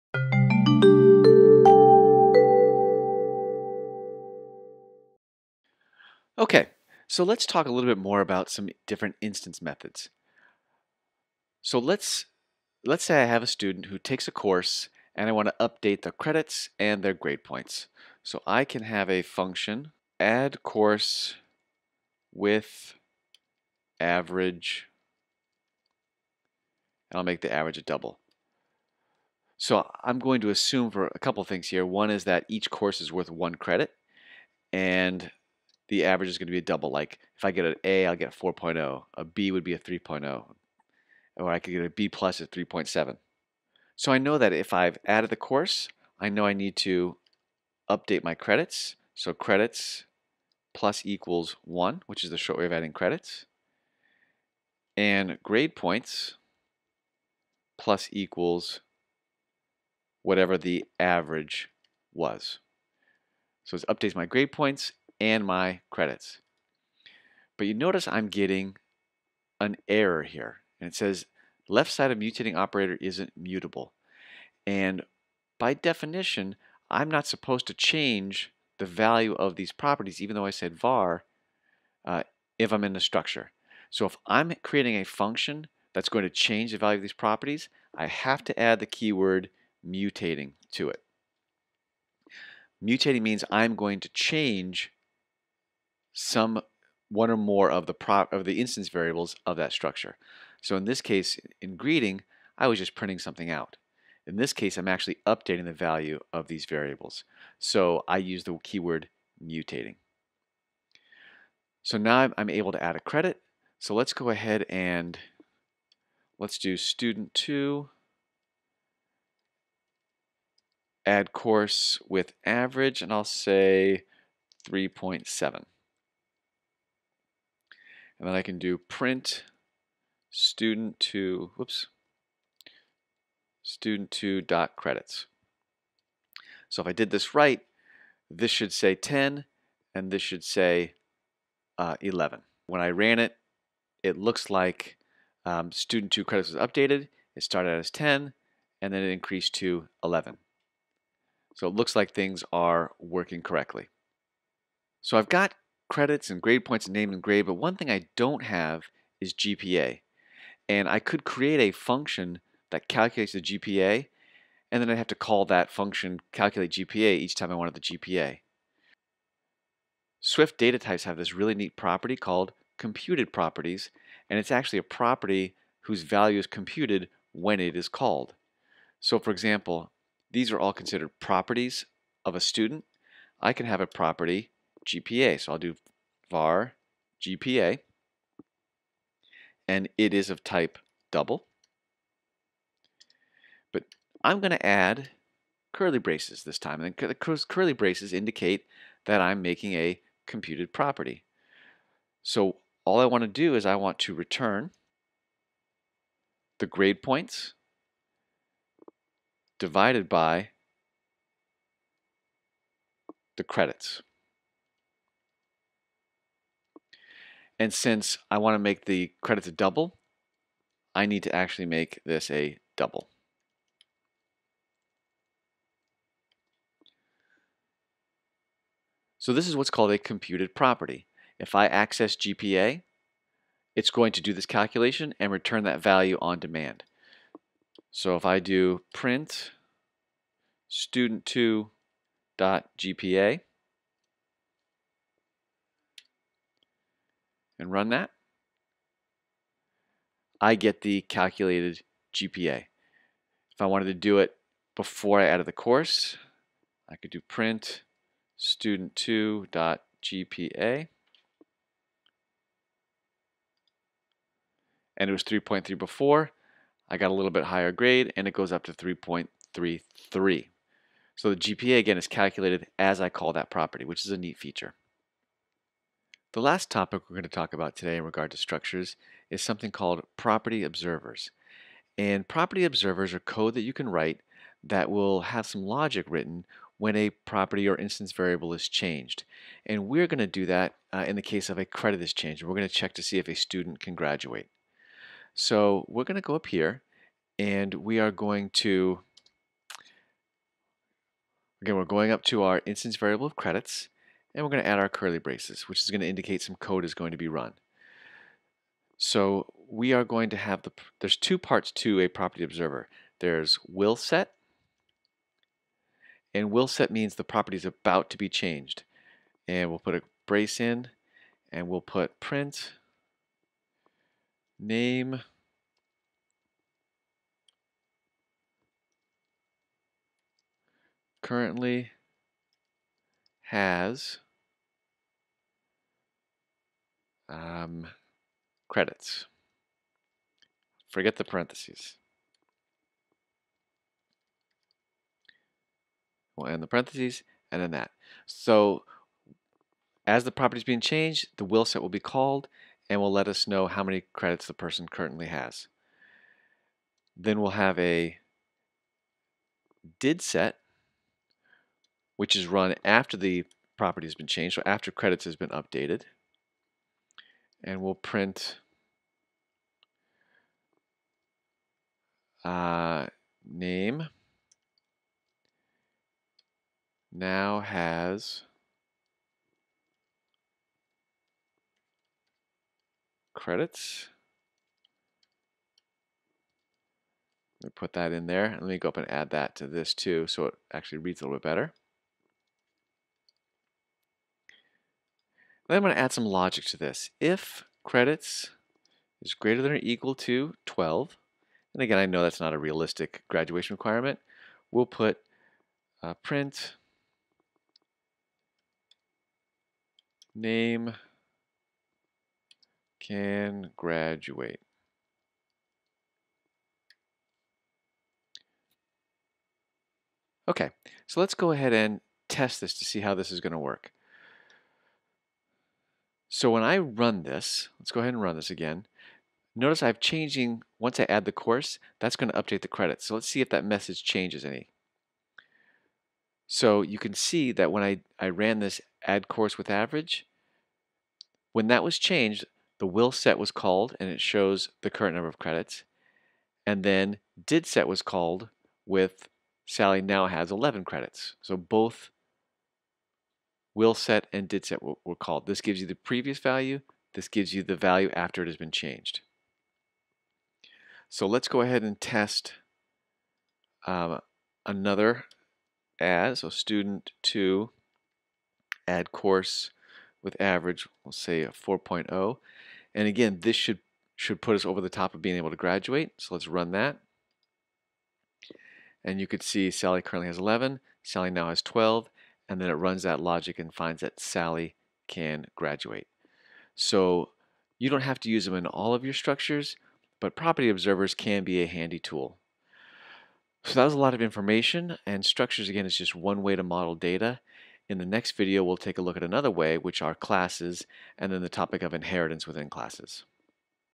Structures Part 2
This video is part of the Introduction to iOS Applications course at Sandy Spring Friends School. In the second part of Structures, I look at mutating functions, computed properties, and property observers.